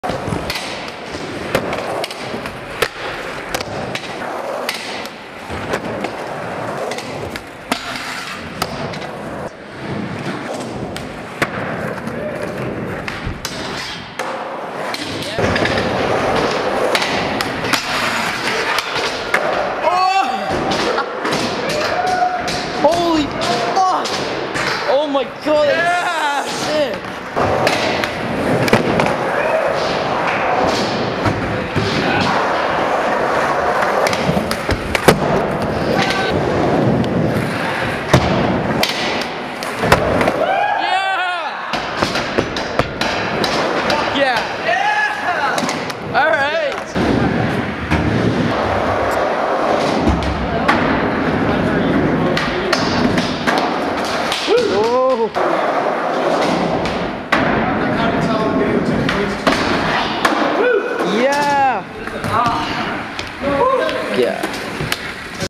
Grady Kuly Edge Skatepark
Grady at the edge for a couple hours